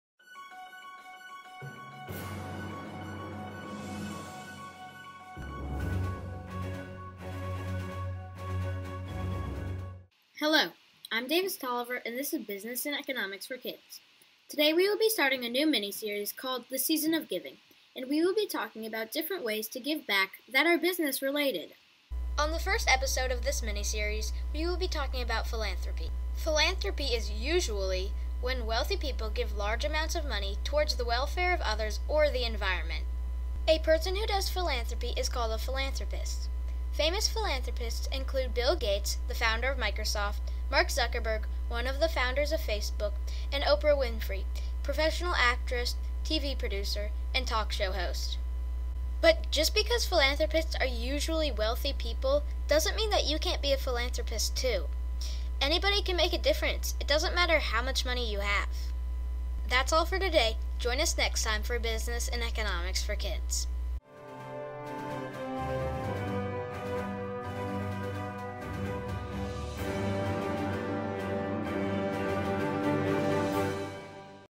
The Season of Giving Episode #1: Philanthropy for Kids
Learn about philanthropy and philanthropists in the kid-friendly first episode of our The Season of Giving miniseries.